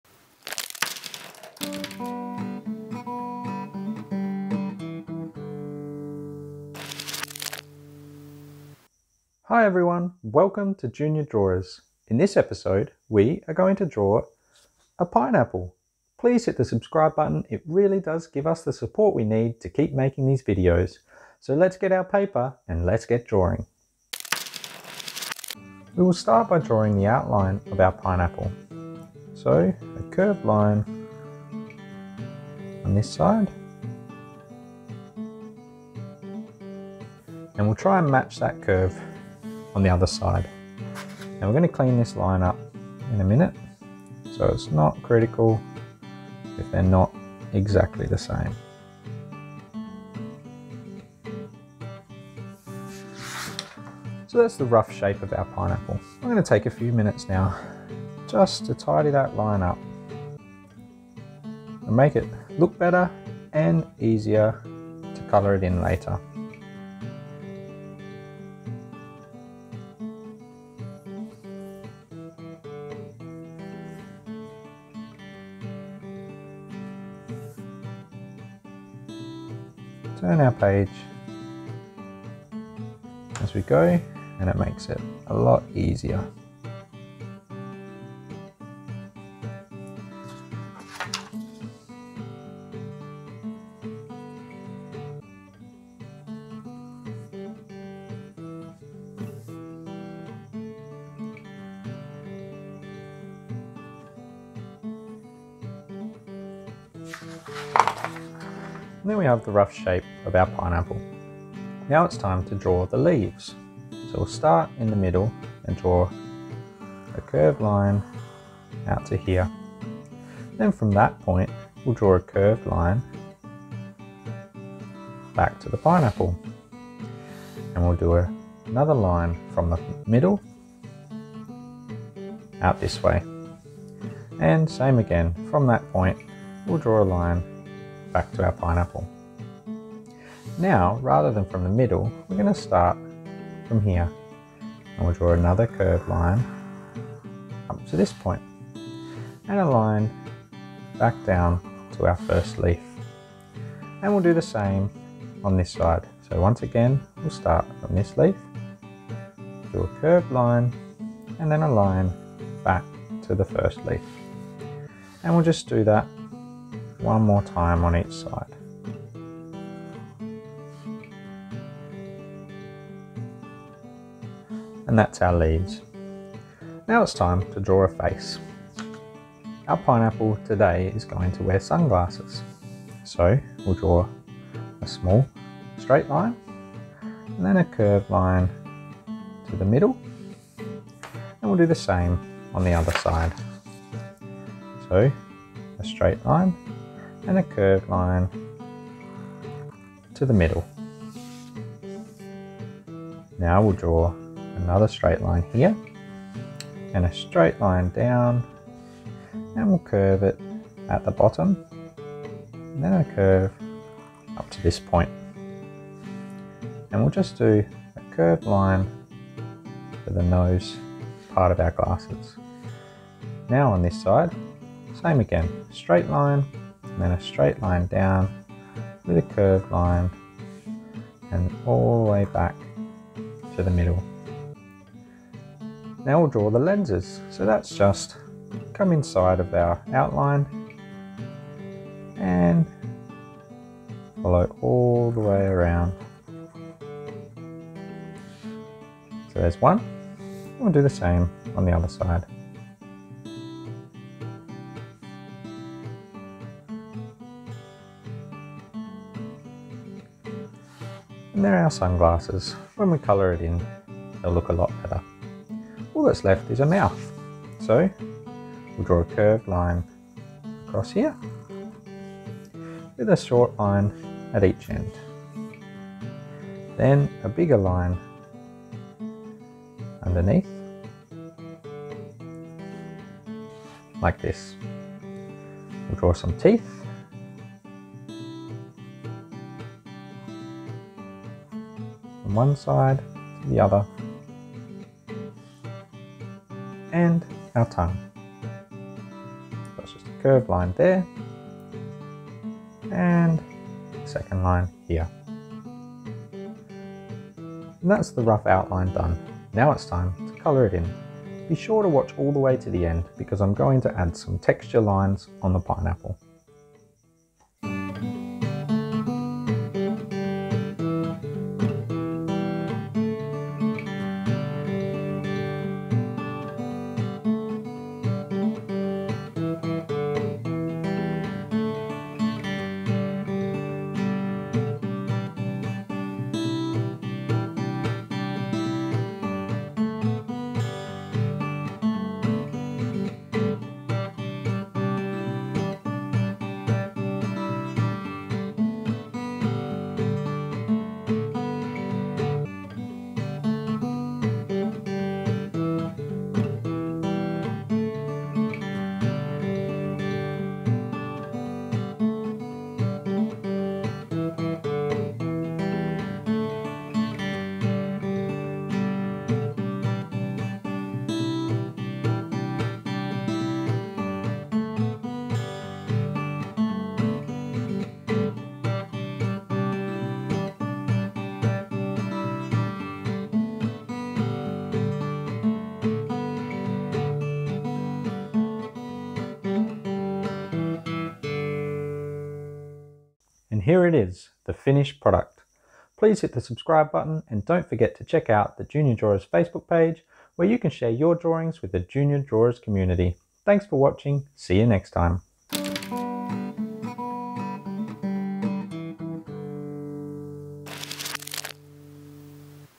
Drawing for kids - How to draw a Pineapple
In this video we will be drawing a cartoon Pineapple.